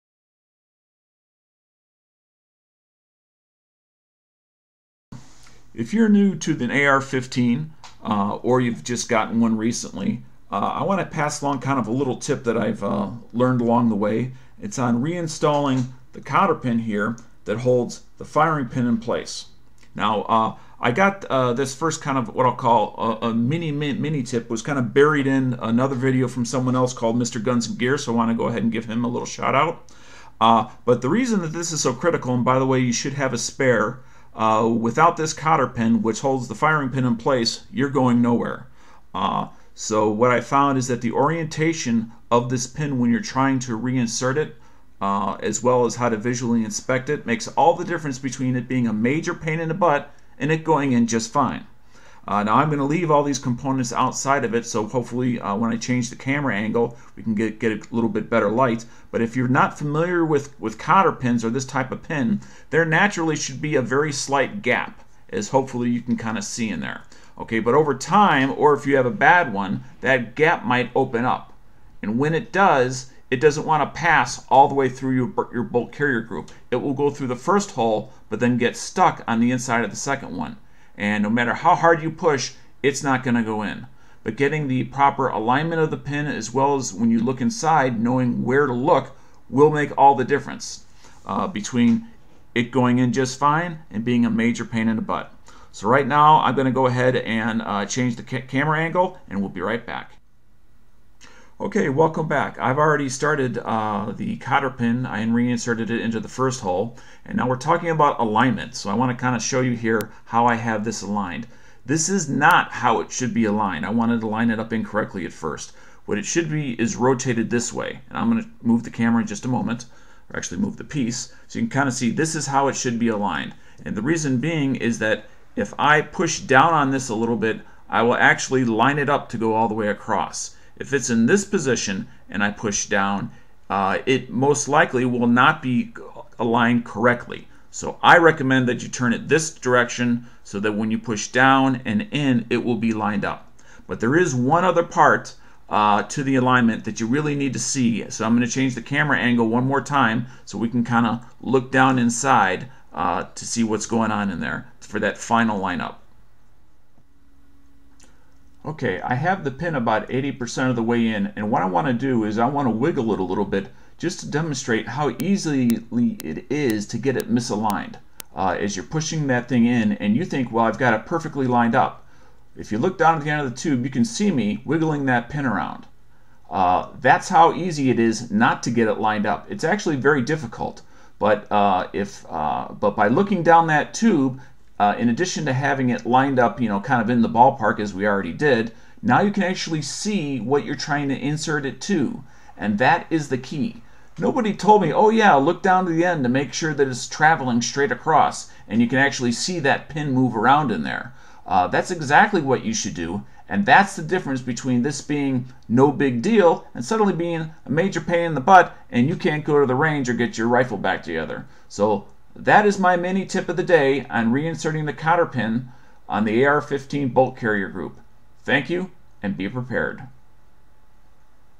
Mini tip AR 15 bolt carrier group COTTER PIN Re-Install toubles ? Take a look
Having trouble getting the cotter pin back into your bolt carrier group? Check out this mini tip.